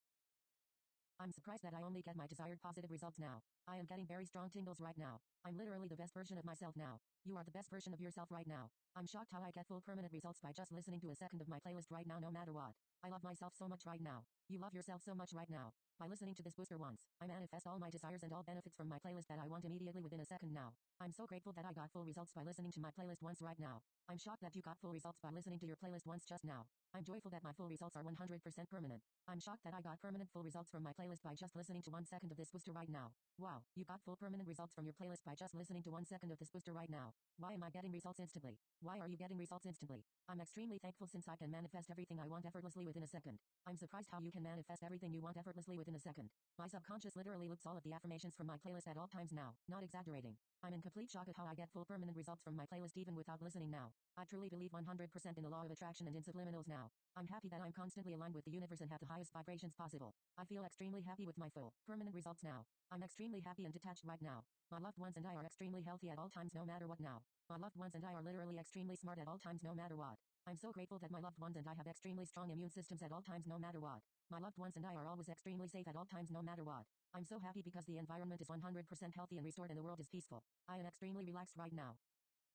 𝐋𝐔𝐗 get full results in 1sec booster silent Ver. 𝙩𝙚𝙖𝙨𝙪𝙪𝙣~[REUPLOAD] Subliminal
This is a re-upload of Teasuun's get full results in 1sec booster sublminal.
This is the silent version that was unlisted.